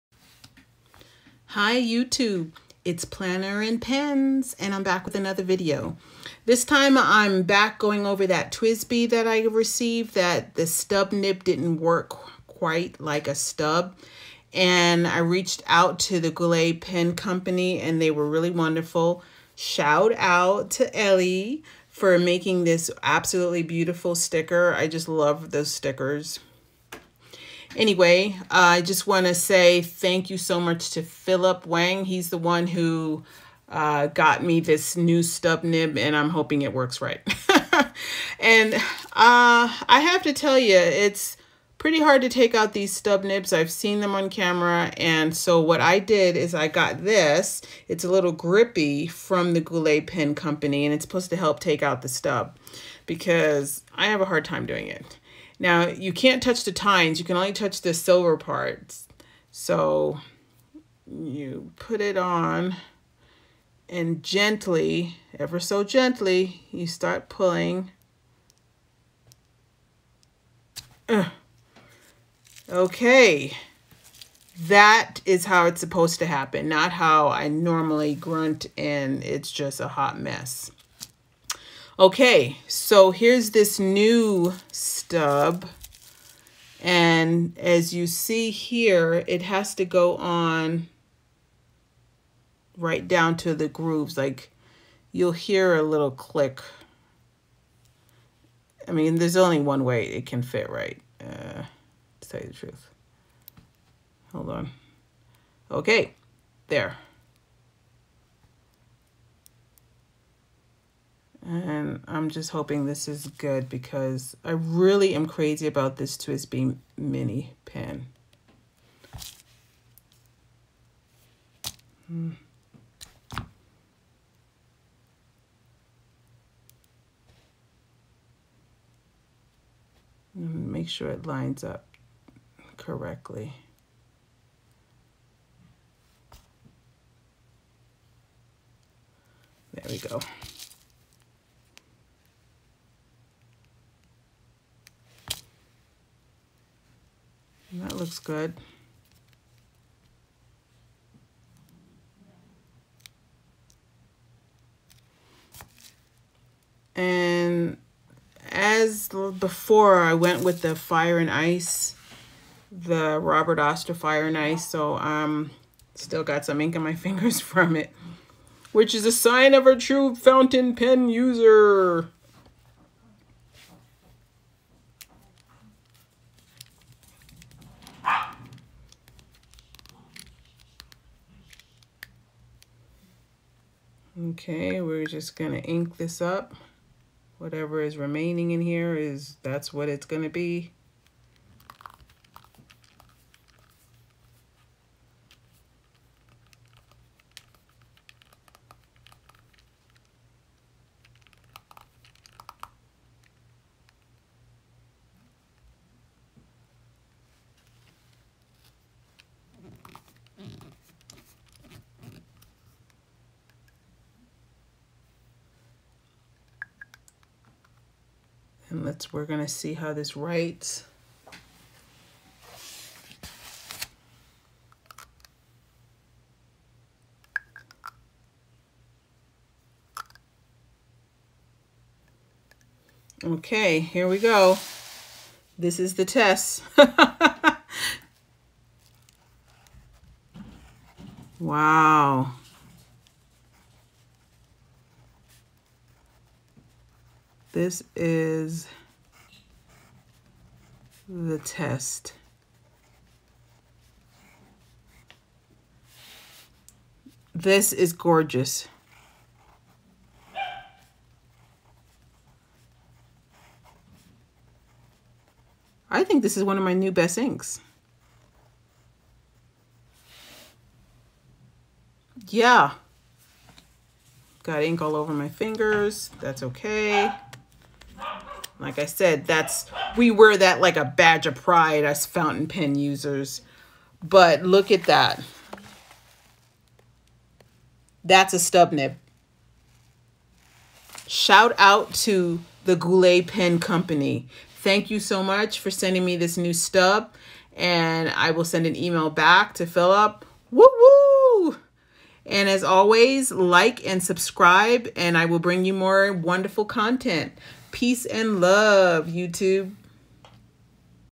Changing TWSBI Stub Nib
I received a TWSBI Mini with a stub nib that wasn't behaving very much like a stub nib. The Goulet Pen Company sent me a new stub nib and watch the video to see what happens.